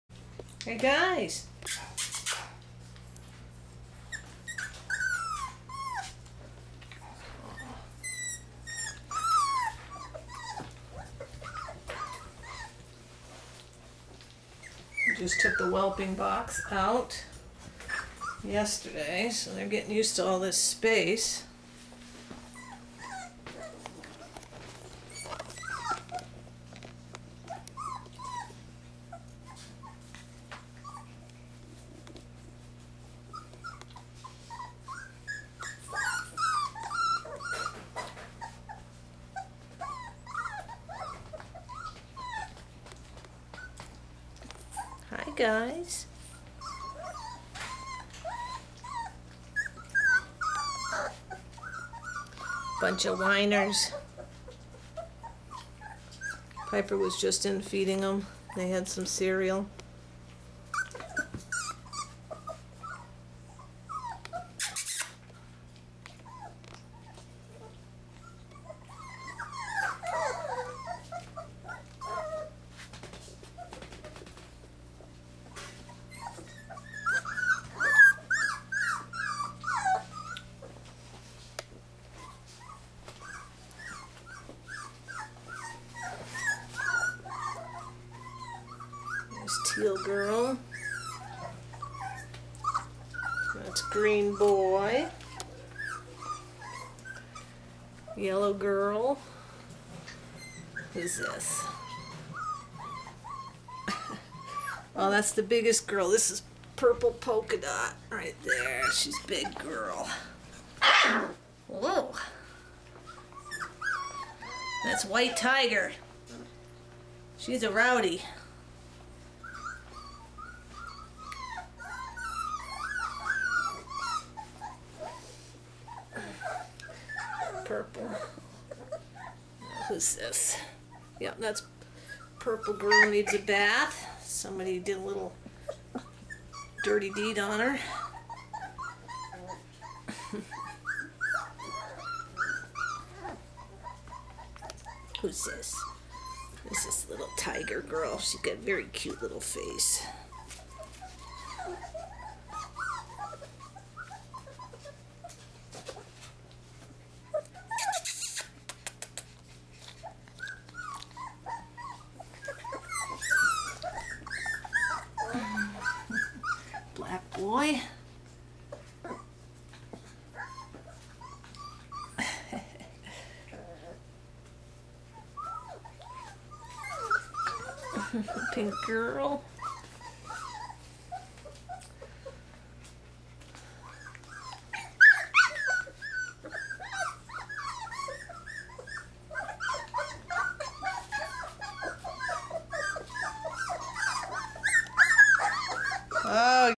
Piper's 2nd pups 6/19/09
This is the pups the day after they got into there new big space, almost 4 weeks old.